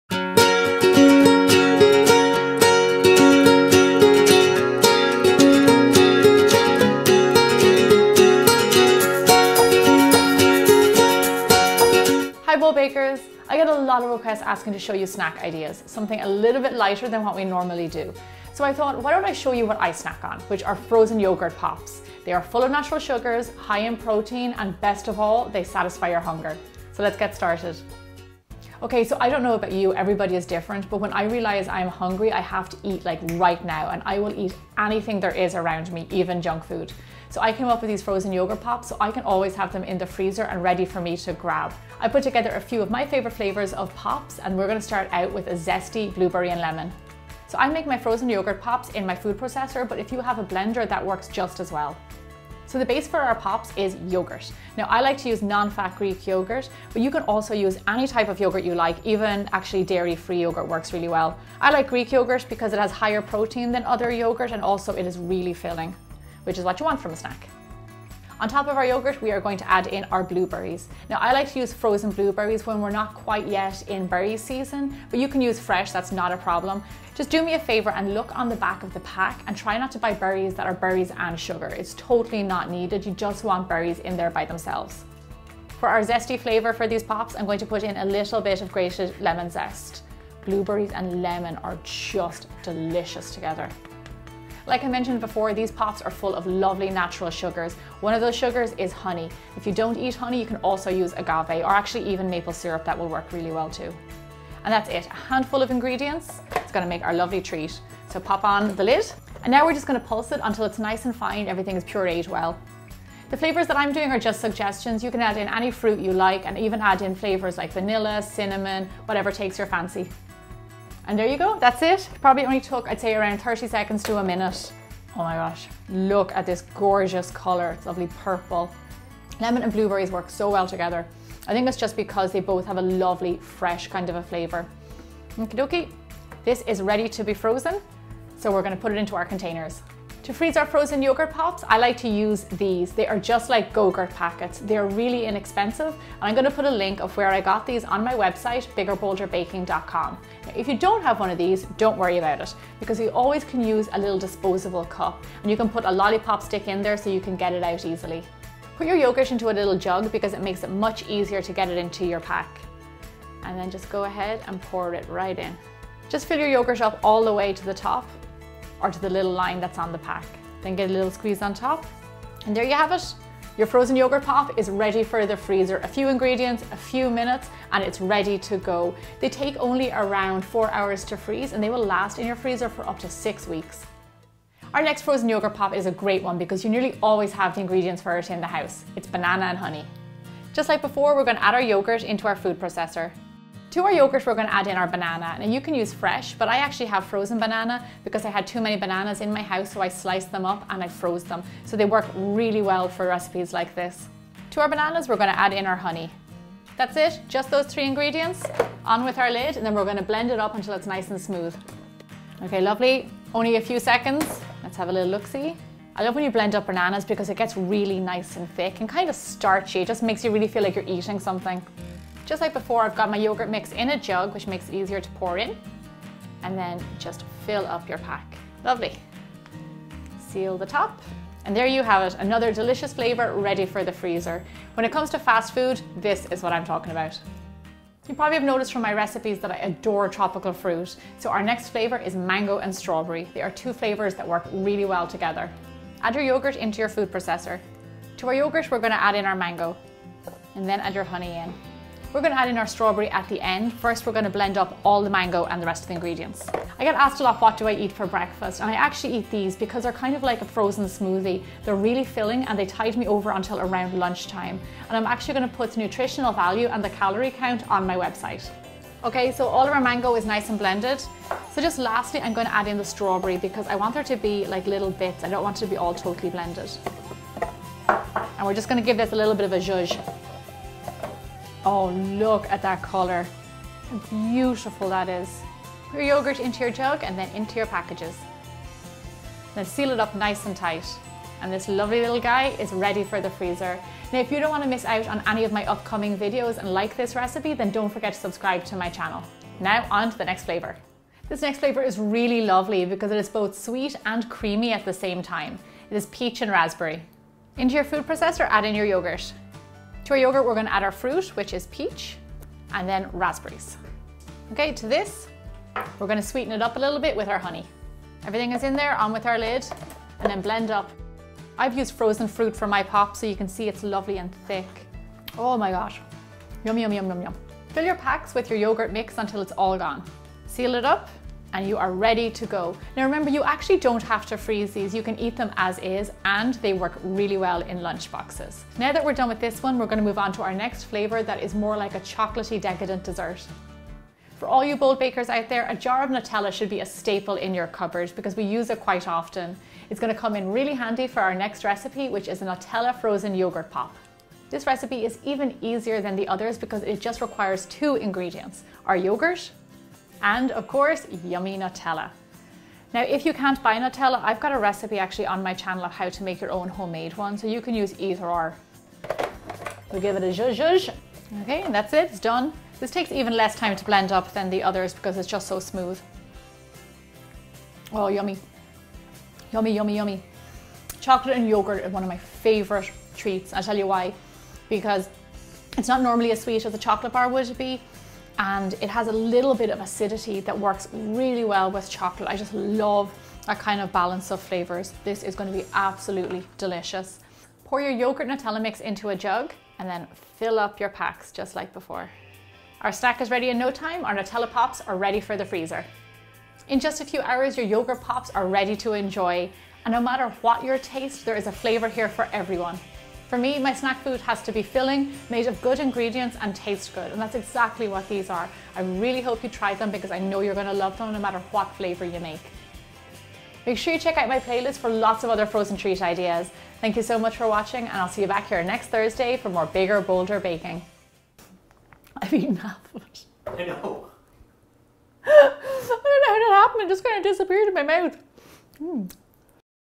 Frozen Yogurt Pops (5 Easy Snacks) Gemma's Bigger Bolder Baking Ep 118
Hi Bold Bakers! I get asked a lot about what I eat for Breakfast and snacks so this week I'm sharing my go-to recipe for Frozen Yogurt Pops. They're like mini-smoothies made with all natural ingredients and frozen into portable snacks. I'll show you how easy it is to make 5 different flavors including Lemon & Blueberry, Strawberry & Mango and even Nutella. So let's get started!